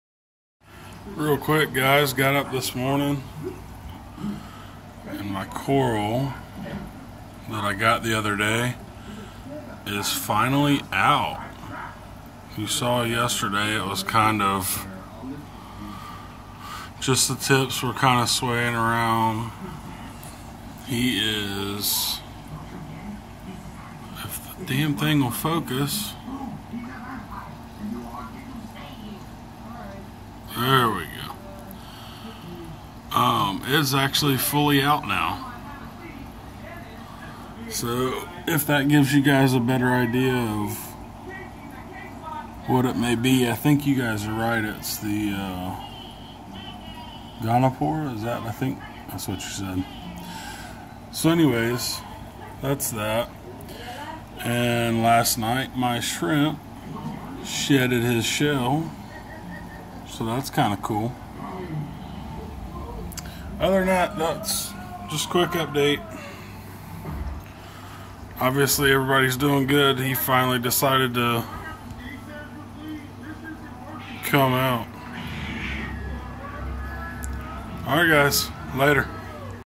My new coral and the shrimp
My coral has busted out finally and the peppermint shrimp shedded his shell